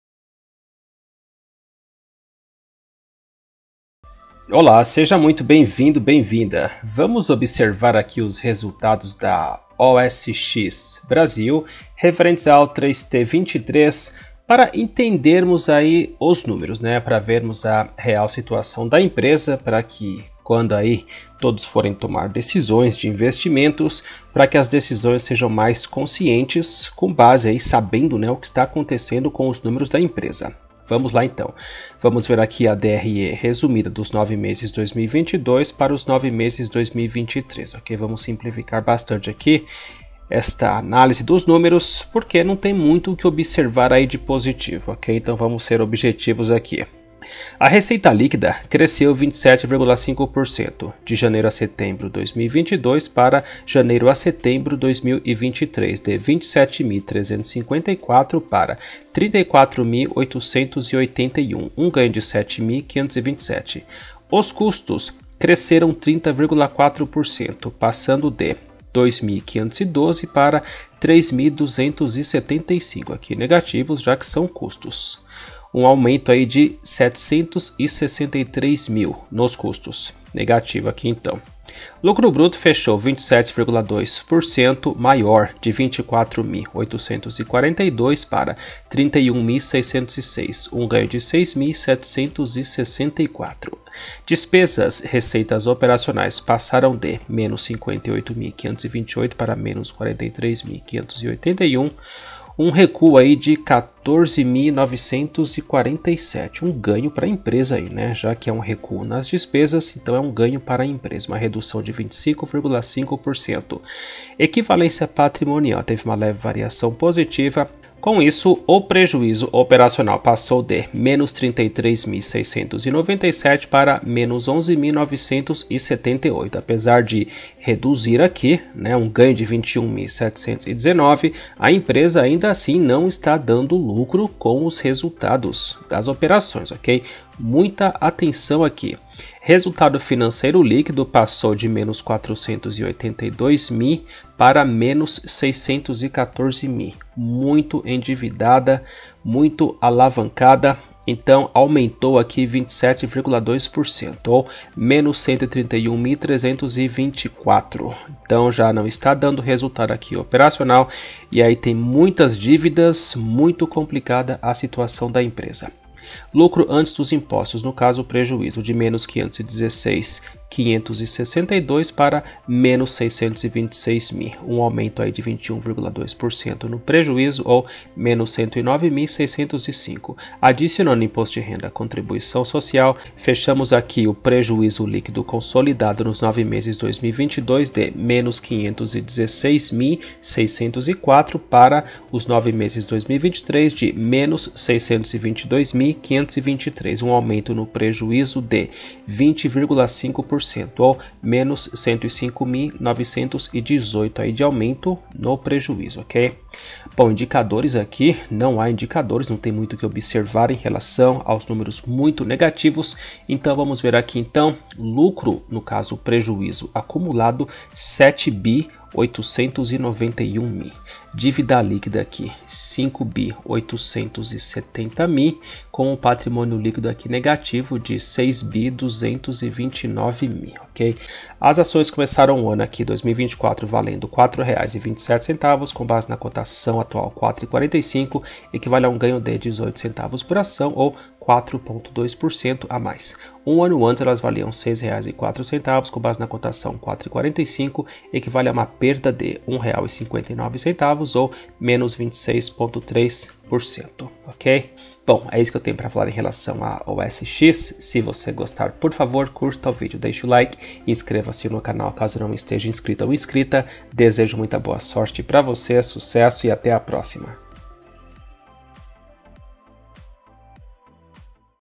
OSX Brasil - Vale Investir Nas Ações? OSXB3 Resultados 3T23 & 9M23 Análise Fundamentalista
Aula Básica Análise de Ações: Indicador P/L (Preço/Lucro) - O que é? Pra que serve? Qual o ideal?